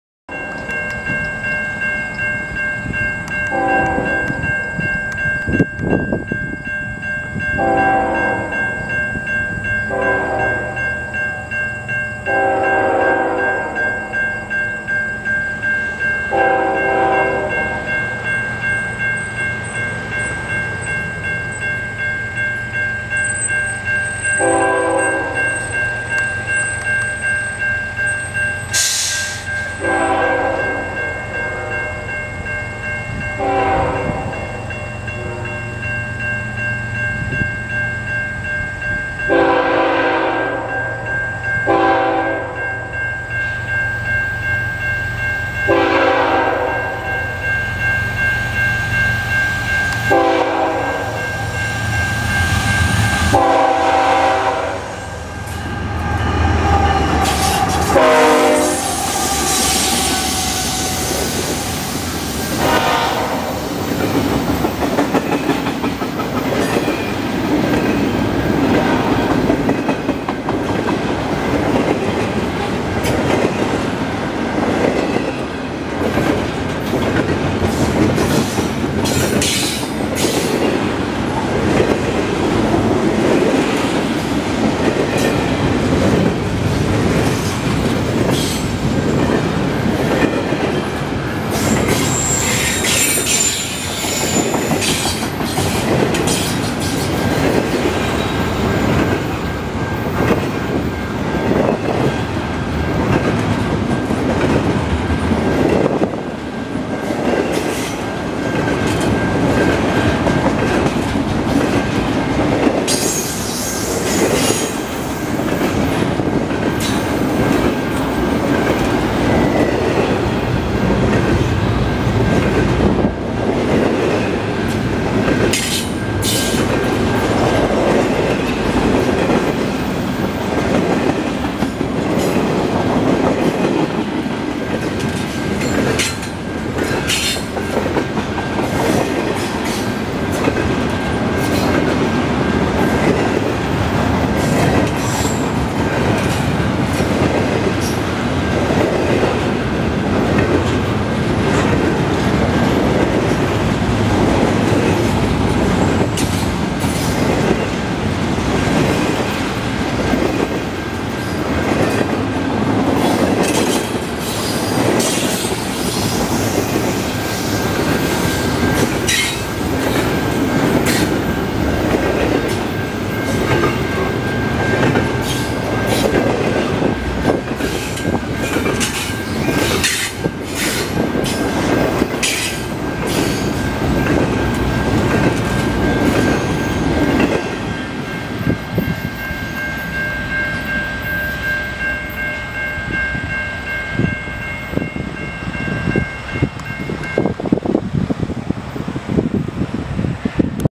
CN 3817, 2825 handle an Eastbound intermodal shipping container in well car train at Vicksburg, MI
Eastbound intermodal shipping container in well car freight train

diesel engine-electric motor locomotives
Evolution Series
ES44AC
CN 3817
ES44AC
CN 2825

DTTX TTX 53' well car
CHINA SHIPPING
tex
COSCO container
1:00
DTTX TTX MAXI-Stack 53' wells car
COSCO SHIPPING container
MAERSK
CMA CGM
CAI
CHINA SHIPPING

DTTX 40' well car

DTTX MAXI-STACK 40' wells car

DTTX all-purpose 53' well car

FEC Florida East Coast Husky-Stack 53' well car

DTTX TTX  MAXI-STACK 40' wells car
ZIM

CP Rail ribbed, three-unit well car

DTTX ribbed five-unit well car

1:30

DTTX three-unit 53' wells car National Steel Car

CP double-stack 53' well car, Gunderson
Beacon container

DTTX 53' well car